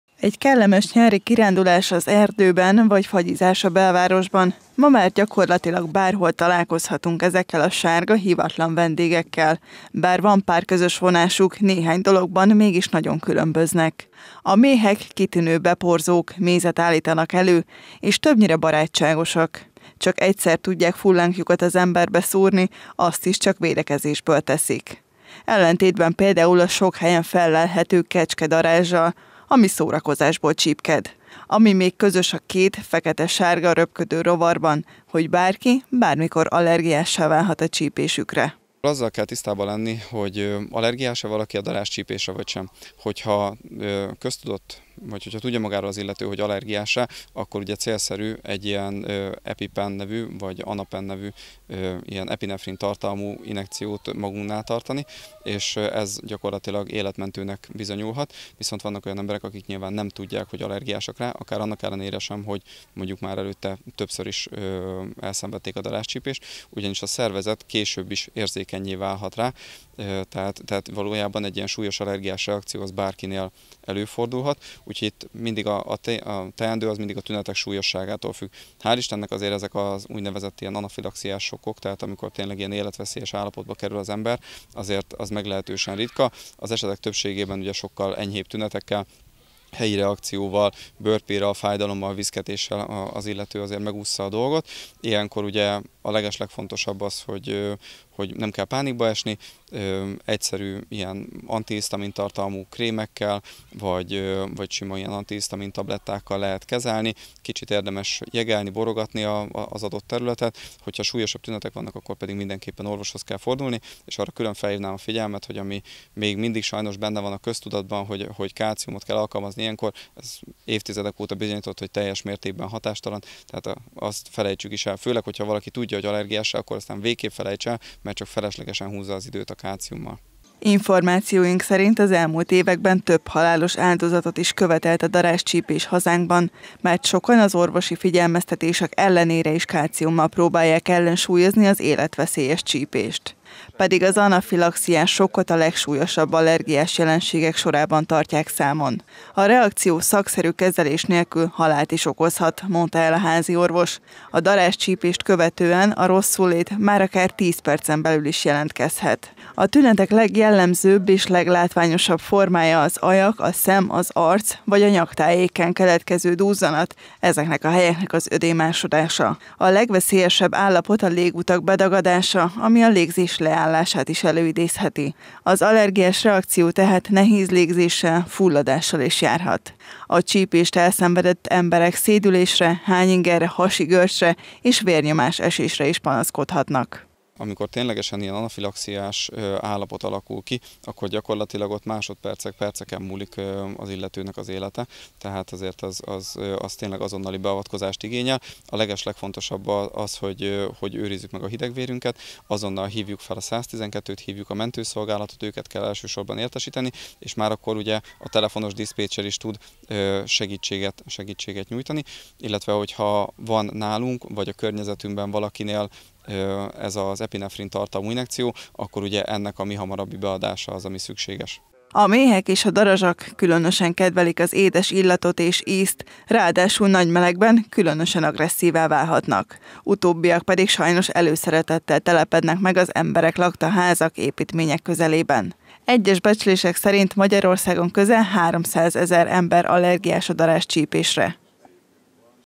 Vigyázz, darázsveszély!
A szokatlanul meleg nyári időjárásban megszaporodtak a házak körül a darazsak. Így gyakrabban fordul elő az is, hogy a házban élőket megcsípi a rovar. Több évtizede megdőlt az a tény, hogy kalciummal orvosolható a darázscsípés. Allergiás reakció vagyis anafilaxiás sokk esetén epinefrin injekciót kell minél hamarabb beadni a comb izomzatába. Az első tünetek általában az allergénnel való érintkezést követő 10 percen belül jelentkeznek. Megfelelő kezelés nélkül viszont halálos is lehet.